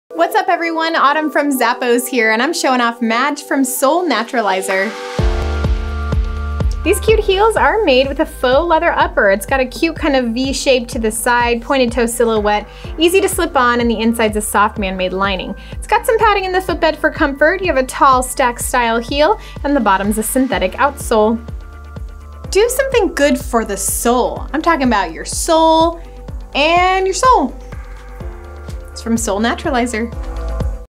SOUL Naturalizer Madge SKU: 9265054
Let your style climb to new heights wearing the  Natural Soul&trade; Madge slip-on bootie.
Featuring a faux-leather upper in a pointed-toe silhouette.
Soft man-made lining and a padded footbed for added comfort.
Stacked heel and a durable synthetic outsole.
Imported.